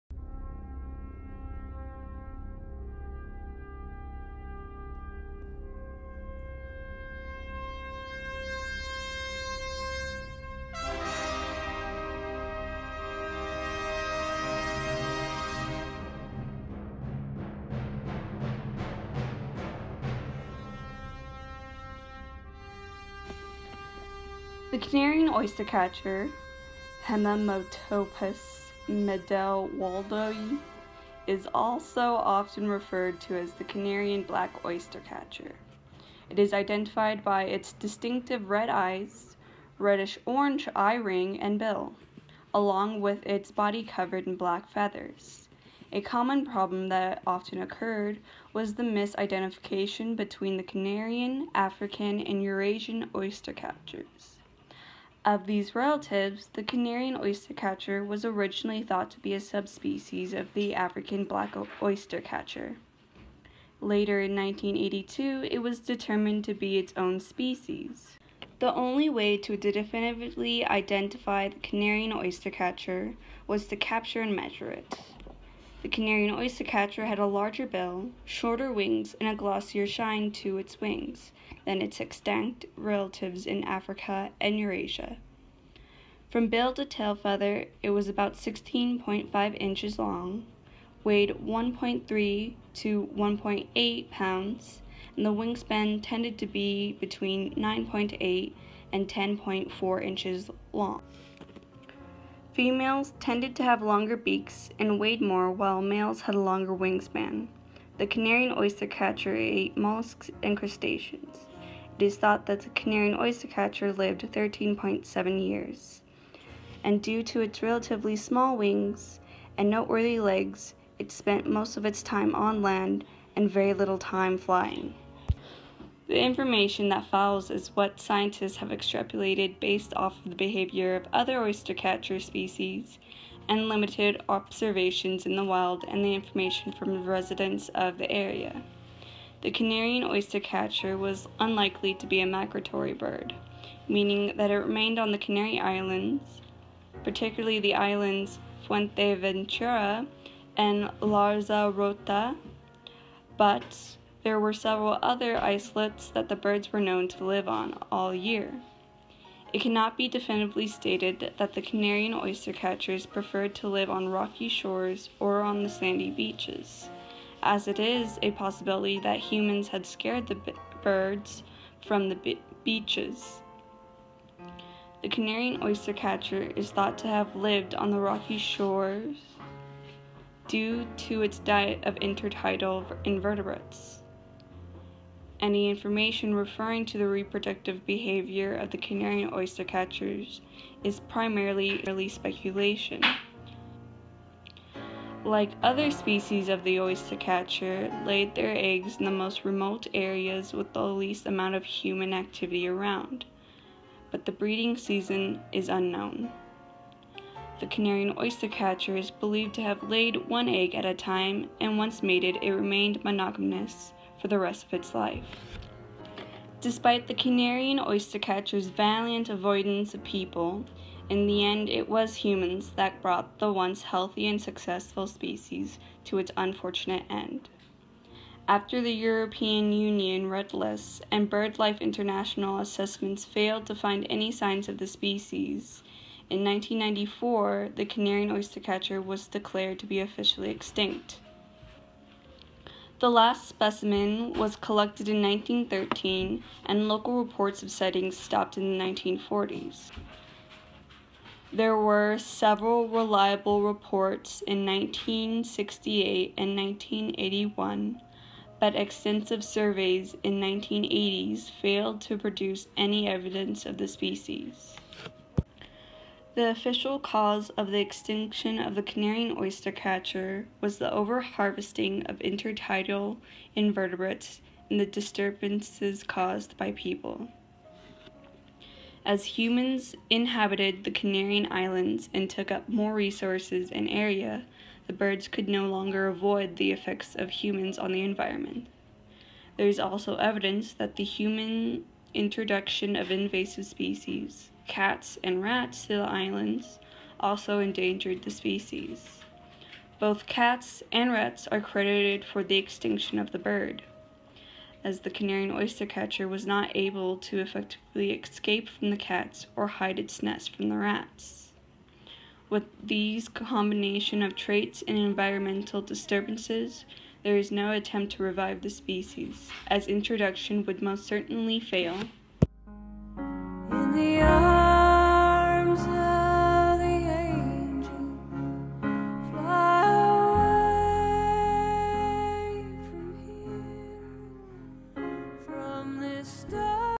Extinction of the Canarian Oystercatcher
i did not mean for the video it self to be so absurd and overshadow the narration.

I apologies to the dogs.